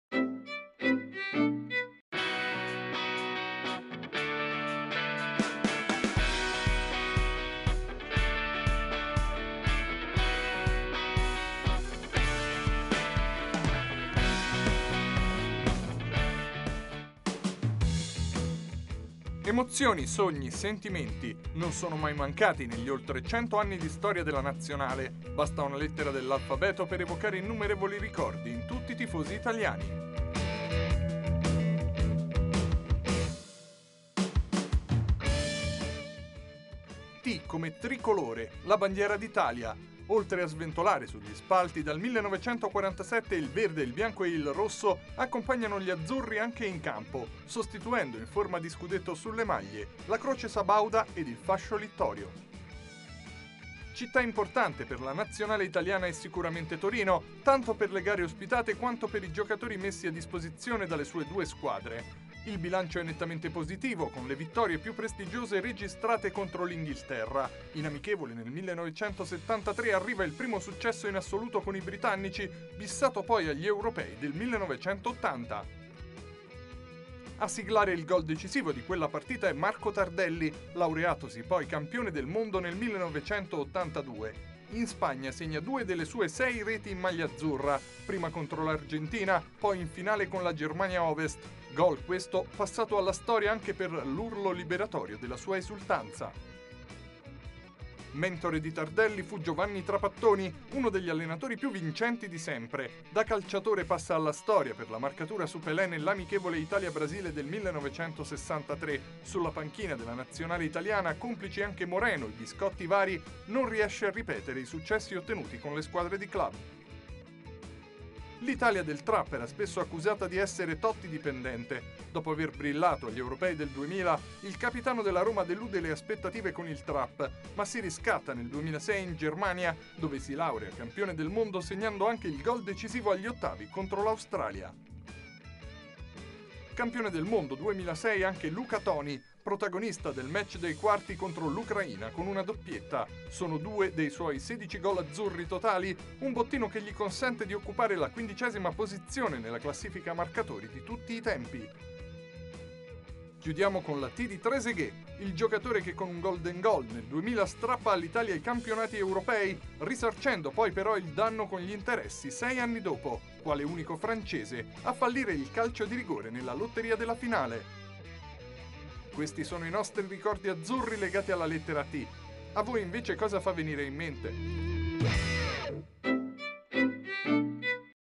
Alfabeto Azzurro: lettera T come Totti, Trapattoni, Torino...
La storia della Nazionale italiana dalla A alla Z